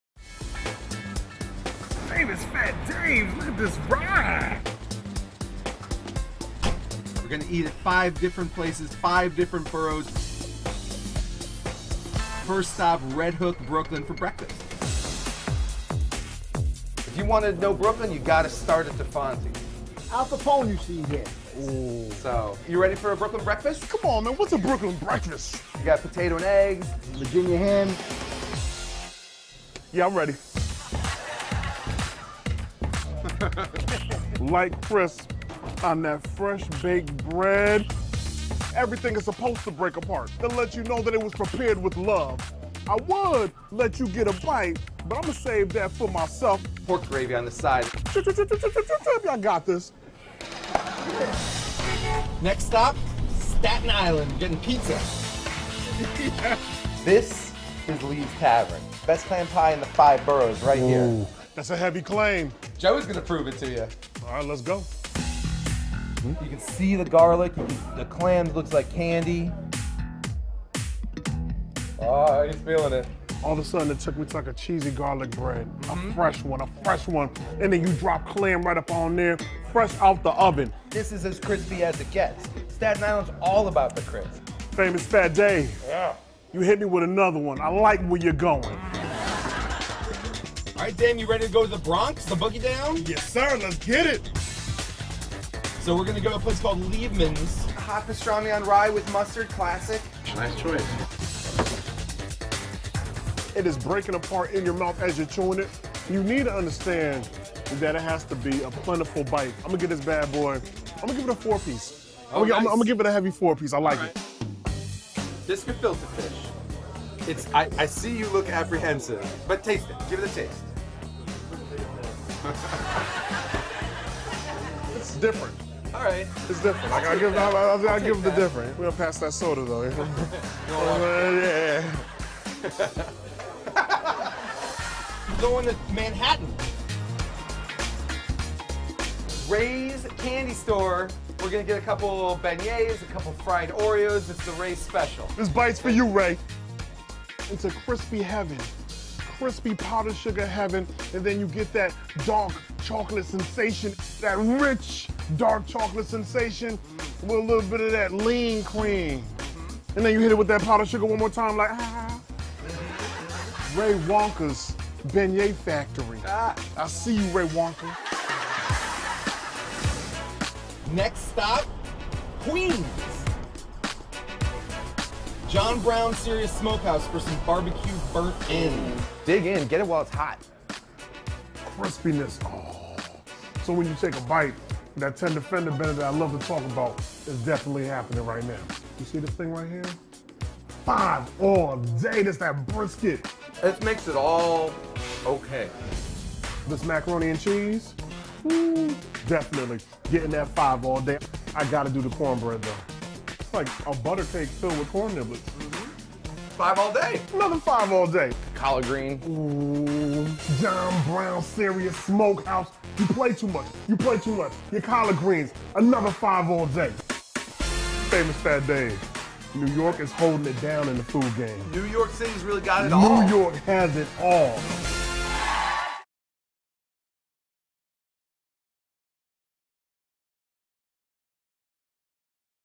Watch Daym Drops Take a Taxi Eating Tour of NYC
When YouTube foodie Daym Drops took a food tour of New York City with Famous Fat Dave (in his vintage taxi cab!), he discovered some of the most delicious creations the Big Apple has to offer.